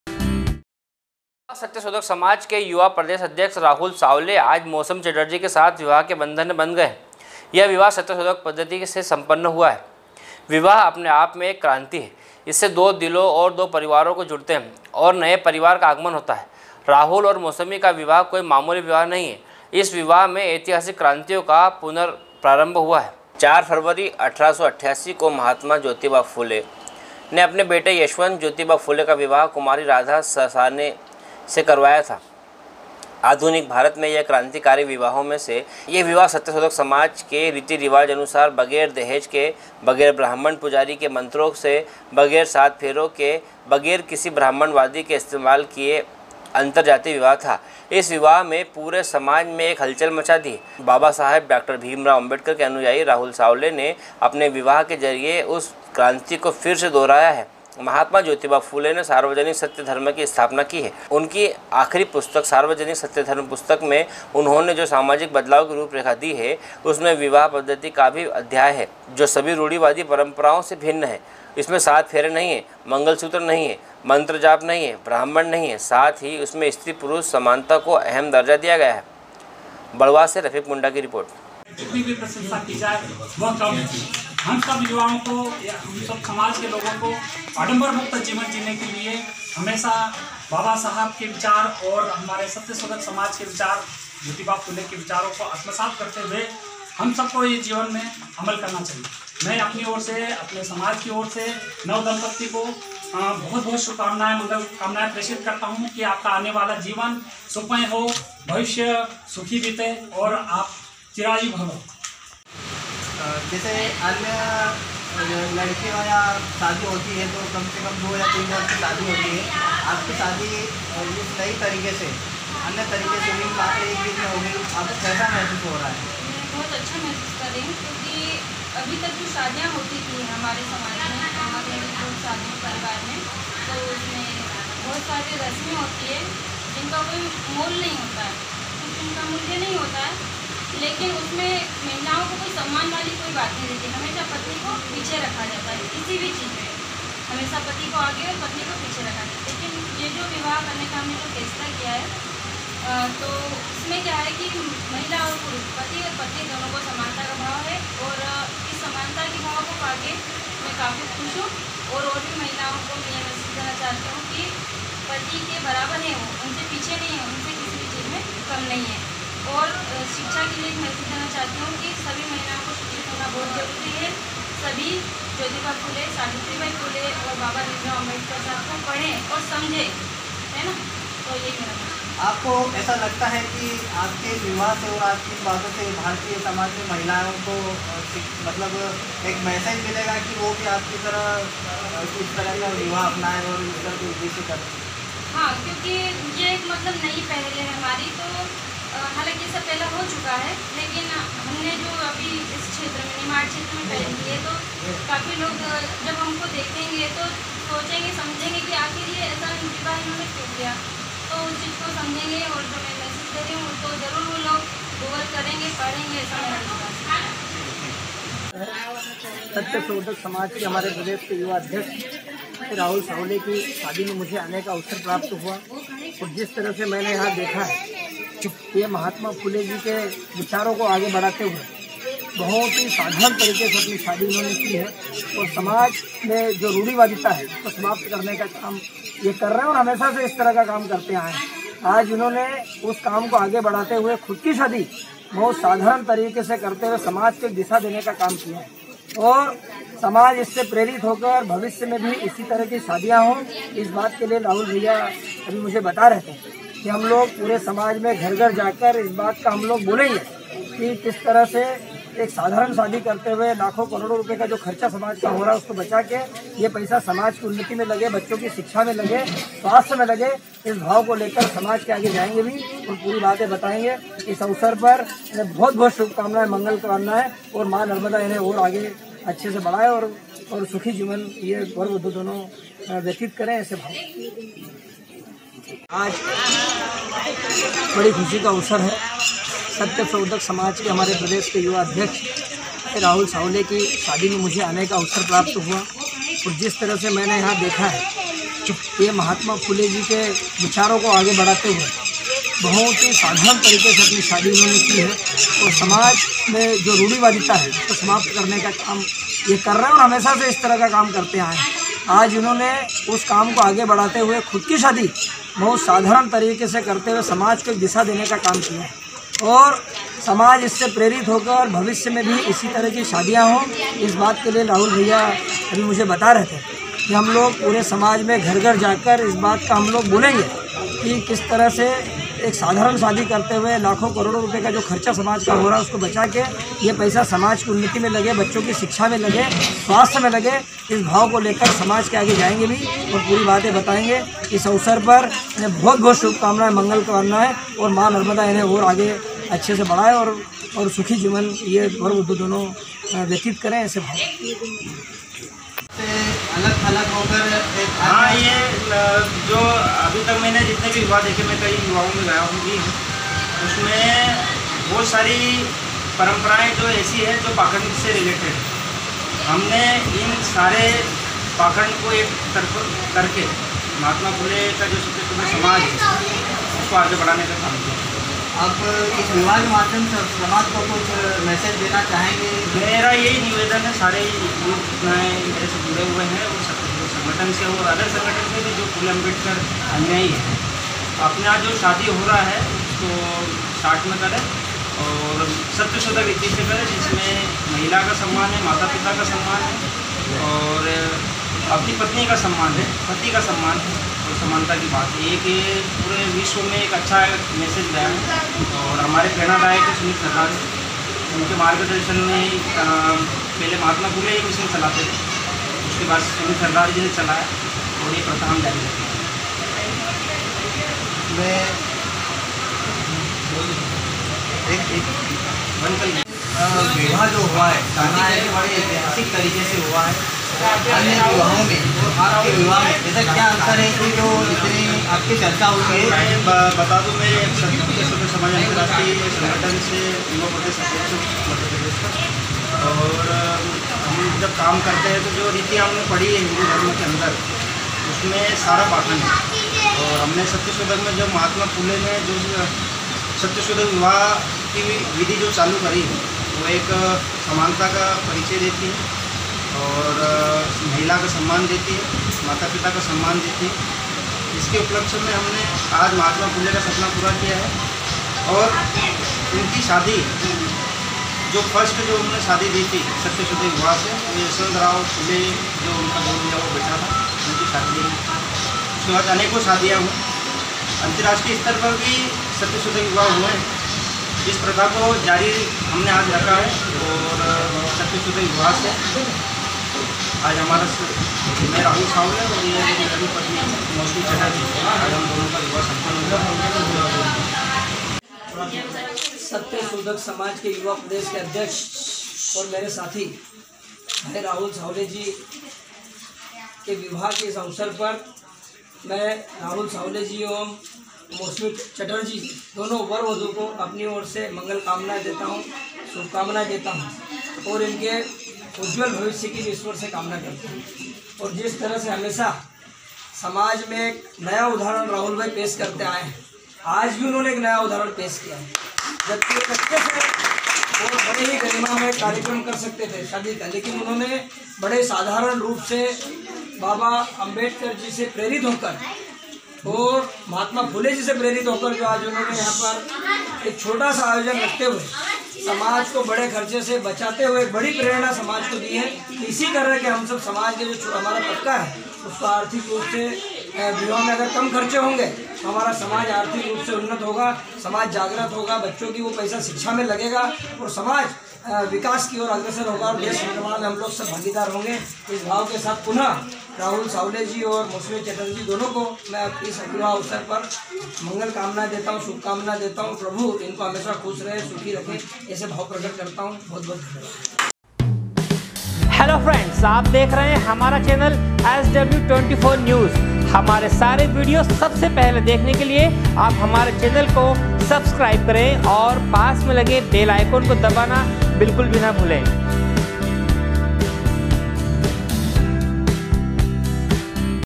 SW 24 News Madhyapradesh#badhwah#11.06.2020
sw sw24news sw24newsmadhyapradesh livenews news24बडवाह सत्य शोधक समाज के युवा प्रदेश अध्यक्ष राहुल सावले आज मौसमी चटर्जी के साथ विवाह के बंधन में बंध गए। यह विवाह सत्यशोधक पद्धति से सम्पन्न हुआ। विवाह अपने आपमें एक क्रांति है, इससे दो दिल दो परिवार जुड़ते है और नए परिवार का आगमन होता है। राहुल और मौसमी का विवाह कोई मामूली विवाह नहीं पर इस विवाह से ऐतिहासिक क्रांतियों का पुनरू प्रारम्भ हुआ ।